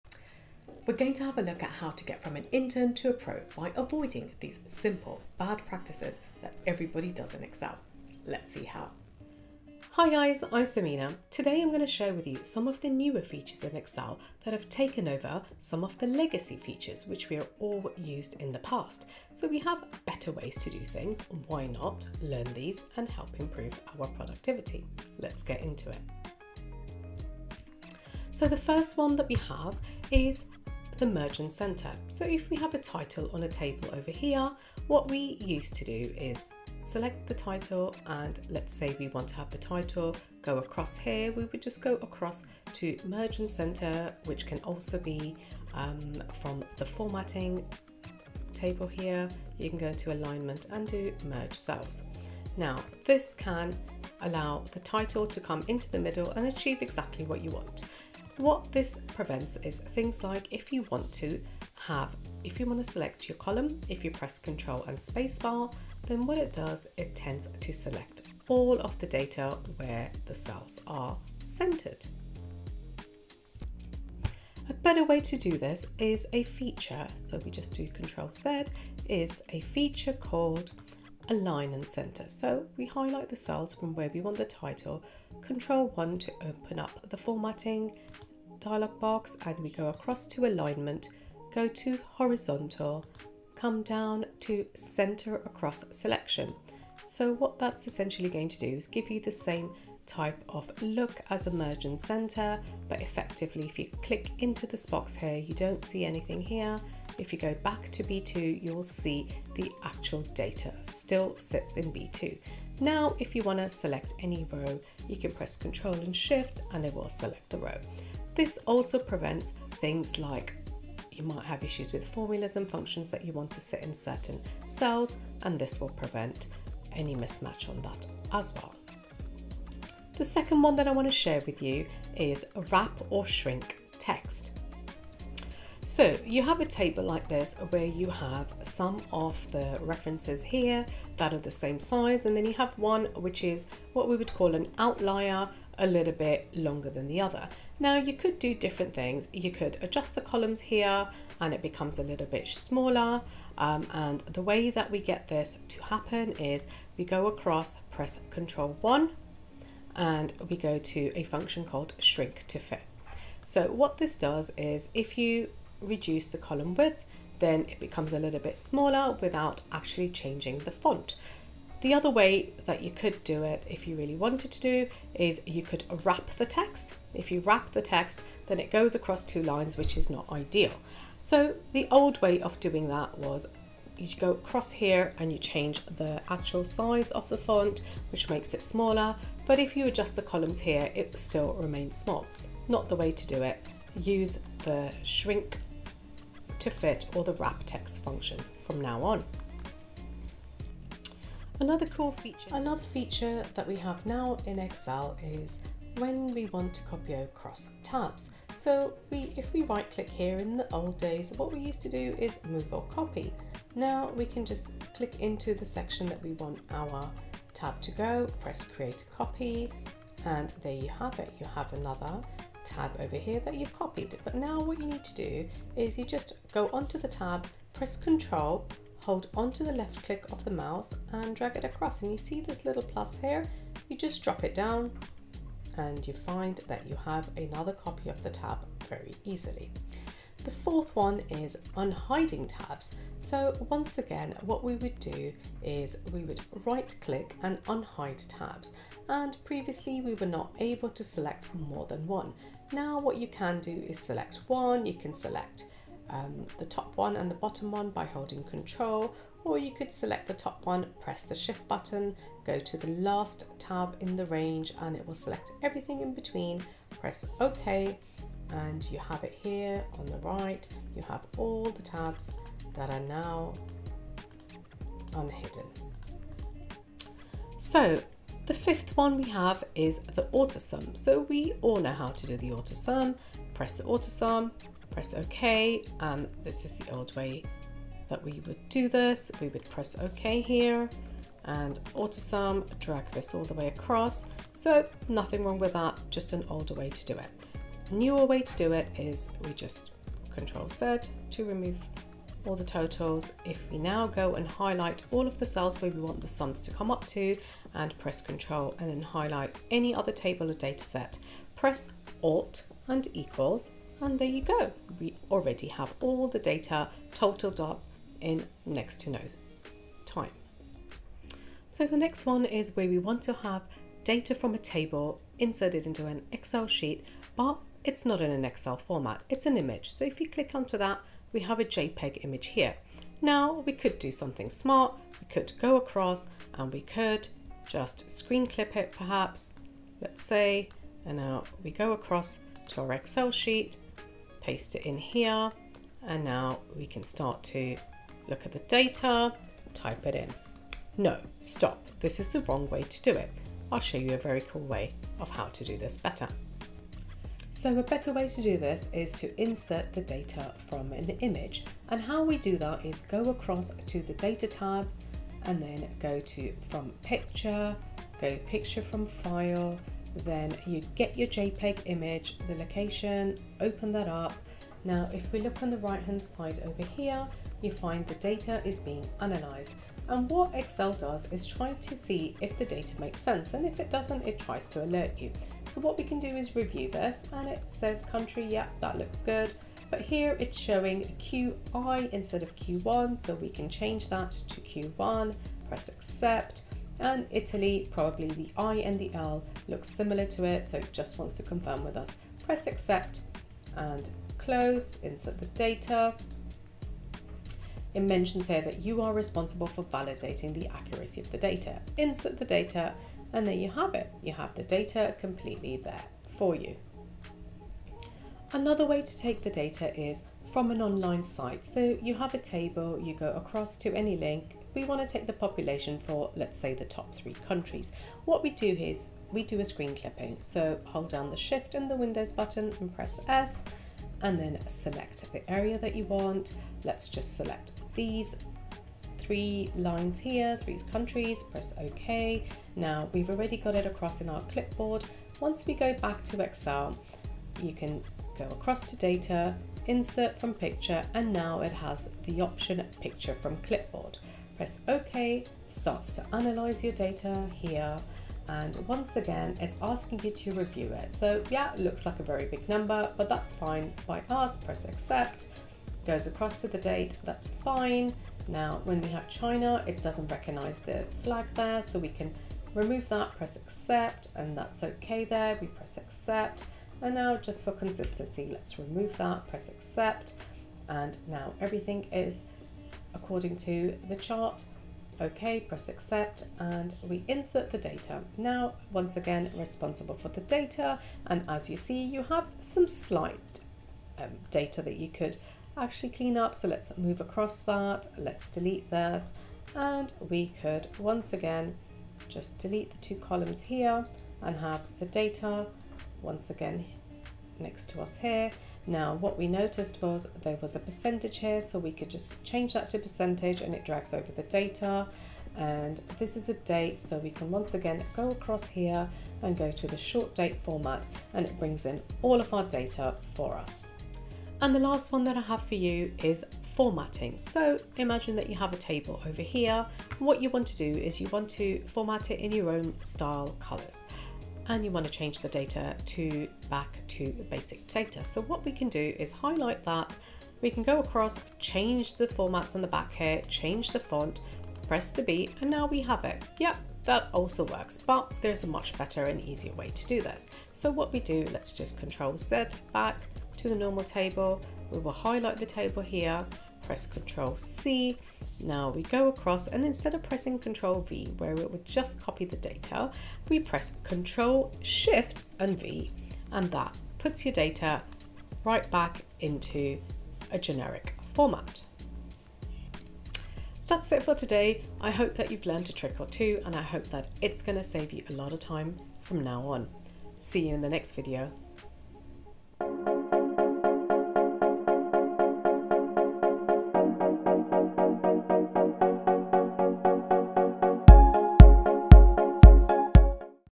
Can You Guess These 7 Common Excel Habits Stealing Your Time?
In this video, we're going to talk about the 7 bad and legacy habits that can prevent you from maximising your productivity. By the end of this video, you'll have a better understanding of to avoid them and use the newer Excel 365 features!

This video will cover:

➡️00:09 1 - Centre Across Selection
➡️02:08 2 - Wrap Text or Shrink to Fit
➡️03:22 3 - CTRL Arrow to copy tabs
➡️04:02 4 - Unhide Bulk Tabs at once
➡️04:38 5 - Autosum many columns & rows at once
➡️05:30 6 - Getting data from an image or screenclip into a table
➡️08:44 7 - Pasting Data while removing formats

**LET’S CONNECT**